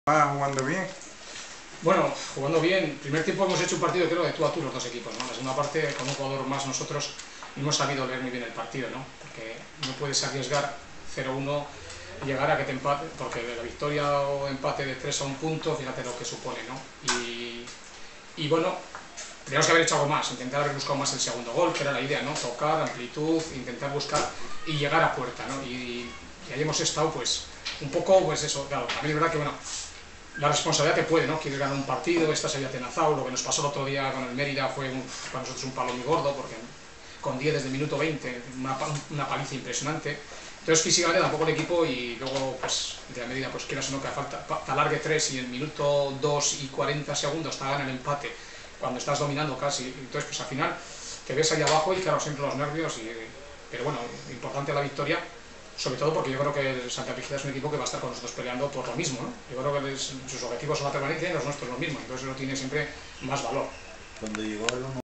(Archivo) Declaraciones de Pachi Bronte tras el partido Villa - CD Alfaro. 28/9/2008
Archive images. Declarations. Spanish football match. Imágenes de archivo. Spanish Second B League. Declaraciones de Pachi Bronte tras el partido UD Villa Santa Brígida - CD Alfaro. 28/9/2008. Temporada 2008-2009. Grupo 2. Jornada 6. Final: 1-2.